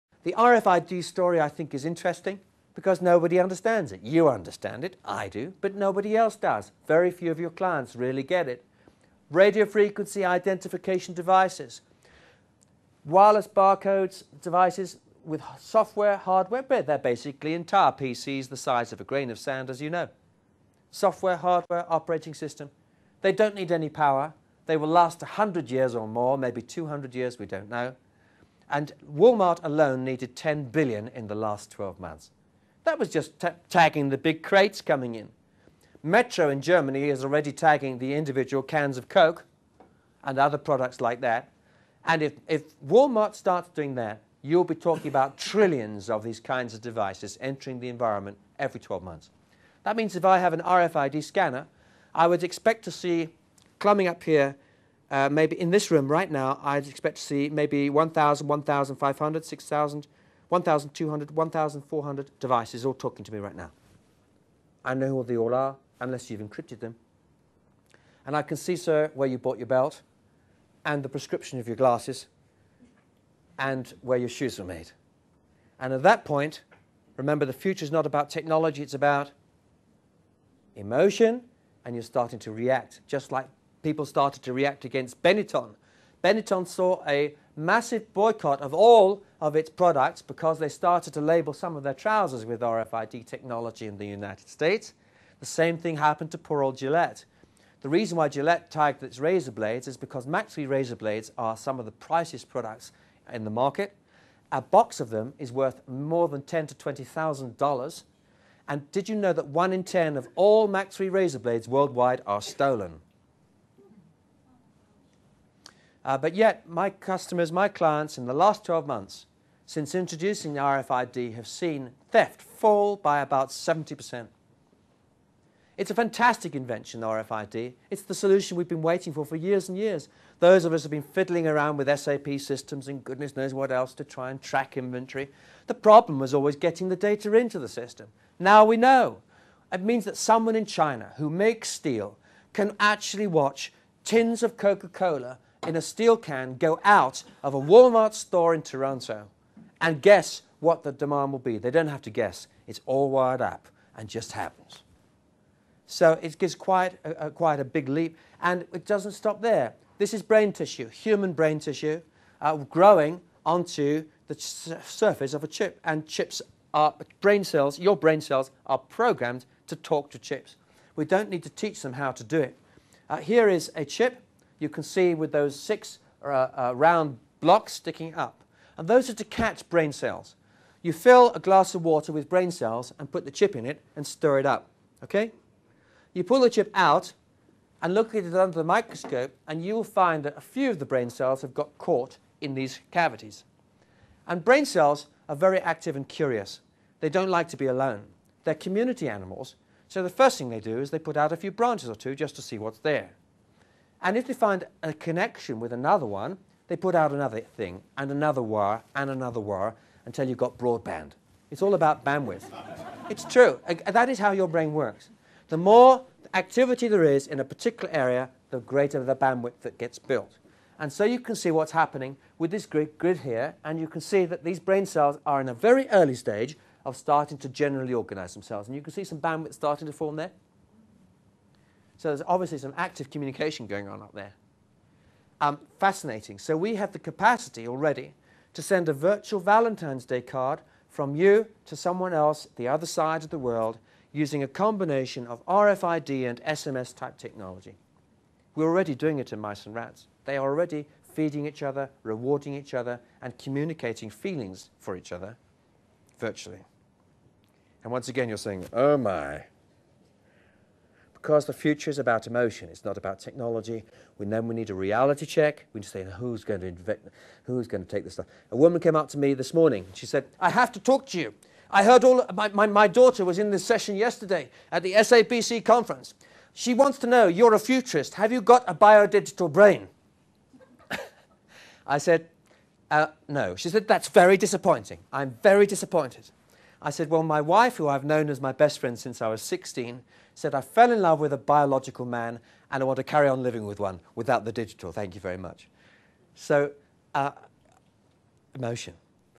100 billion computers the size of grains of sand - RFID chip and how they will change how we live, m
Billions of intelligent devices are being released into the environment, each size of grain of sand, with software, hardware, operating system, memory, wireless communications... as replacement for traditional barcodes.  WalMar aleady insists on all boxes being tagged with these RFIDs in warehouses.  These chips can also be fused with brain tissue enabling animals to transmit basic thoughts. Human beings are already tagging themselves - to get admission into night clubs for example.  That's all in the past.  What about the future?  Dr Patrick Dixon Futurist seminar on technology trends for Dimension Data.  Patrick Dixon is an international conference speaker and author of 12 books including Futurewise.